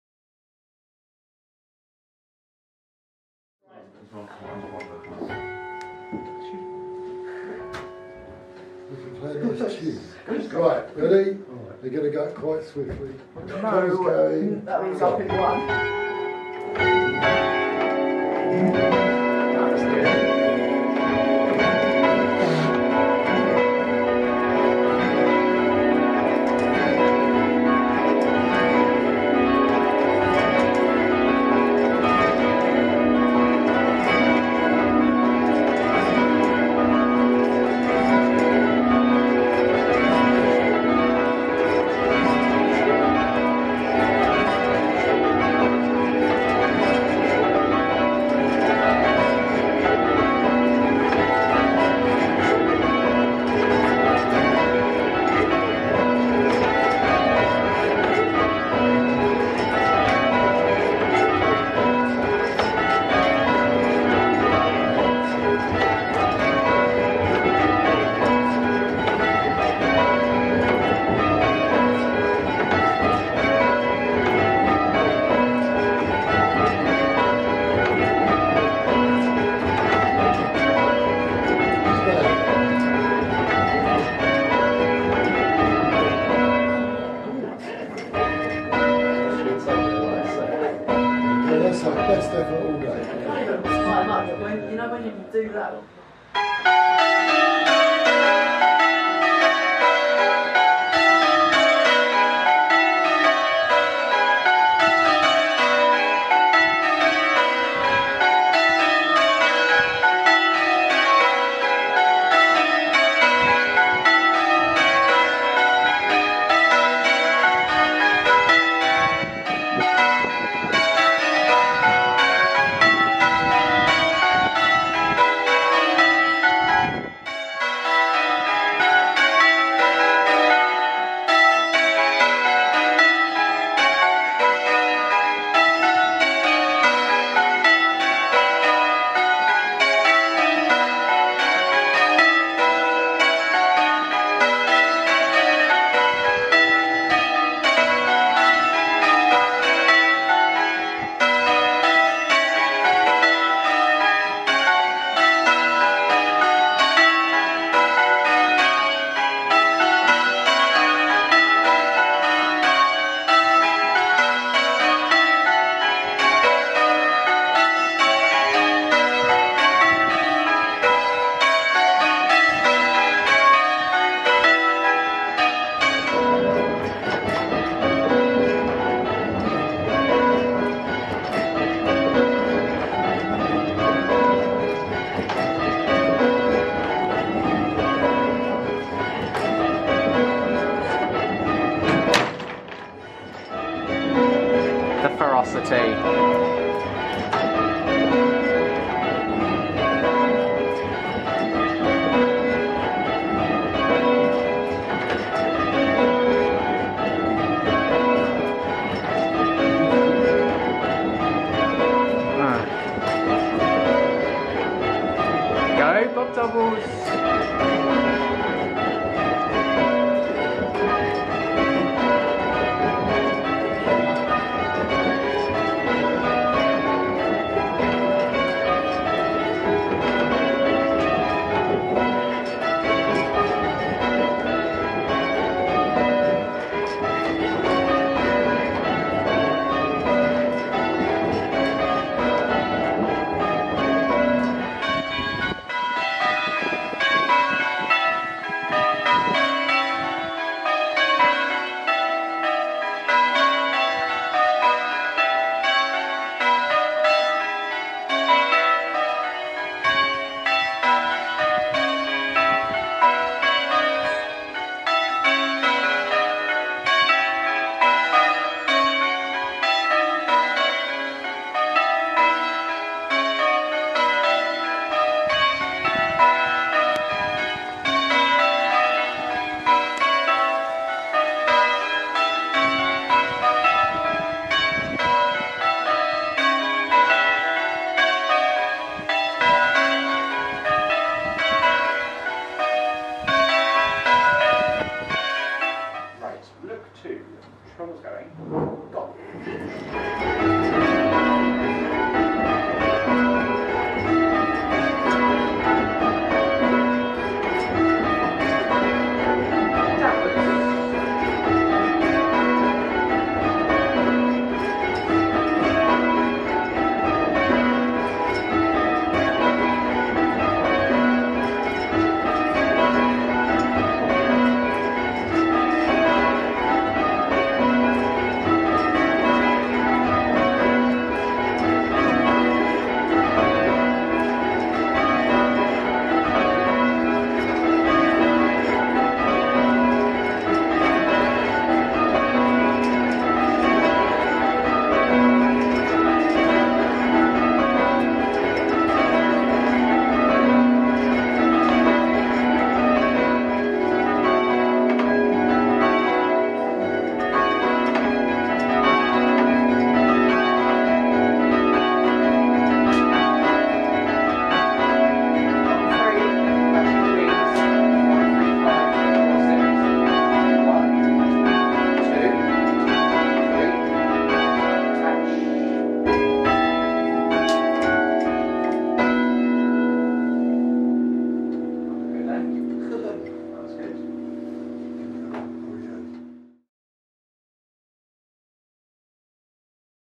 Bell Ringing at Drayton St Leonard, Oxon
Ringing on the lovely ring of six hung in the tower of St Leonard & St Catherine church in Drayton St Leonard. They were hung by Whitechapel in 2016 and were the final ring to be hung by them before their closure. They certainly went out on a high note. Enjoy!

St Leonard & St Catherine, 6 bells, 6-0-19cwt in B.